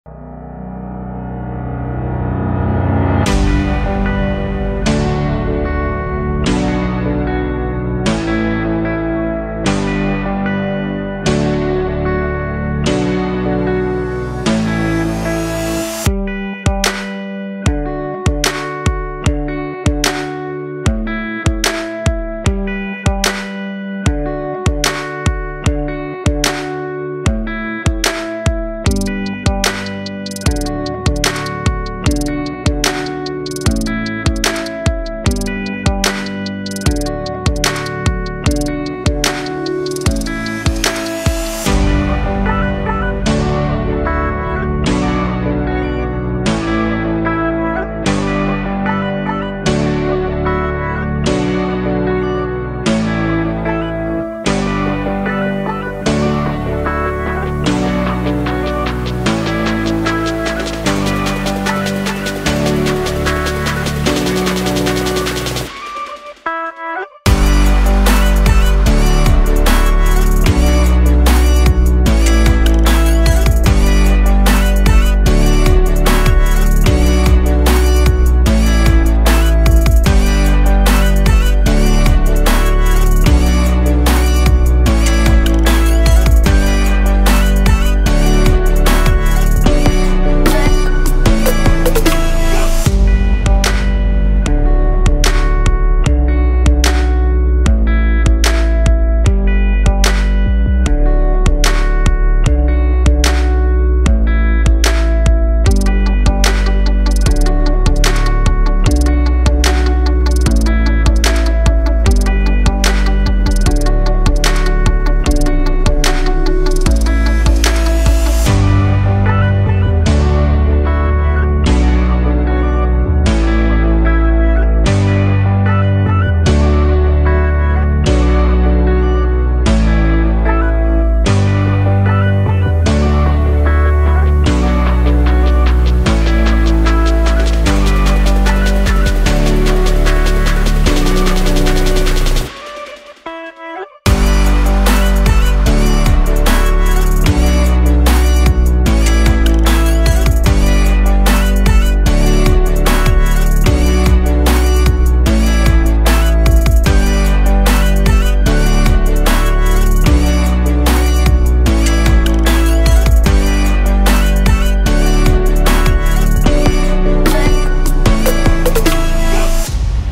Formigas trabalhando em equipe pra carregar algo grande.
Filmei  este  vìdeo  só  pra mostrar  como  que as formigas apesar de bem pequenas  conseguem  trabalhar organizadas.  Neste caso  carregando algo  pesado pro tamanho  delas.  uma bolacha  grande para o tamanho delas .  Formigas tem muita força. Estas formigas do vìdeo não foram machucadas e conseguiram levar a bolacha pra perto do  ninho.